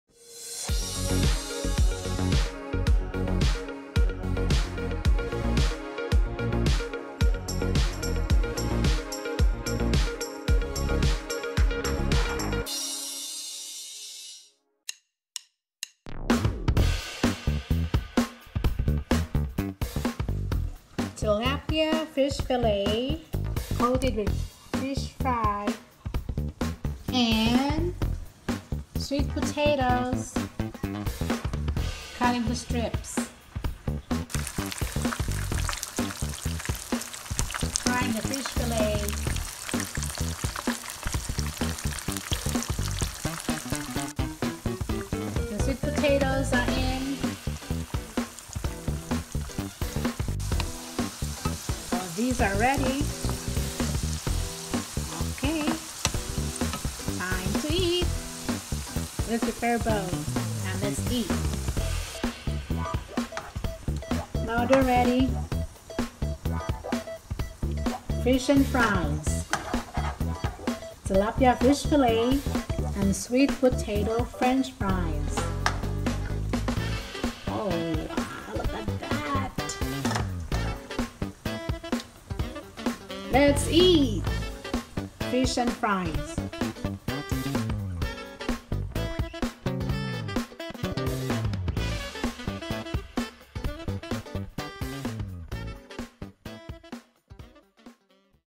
Fish and Fries | Tilapia Fish Fillets and Sweet Potato Fries
We love fish and fries, and I want it a little bit healthier , so I'm using sweet potatoes (camote) for fries. They're delicious! Take a look!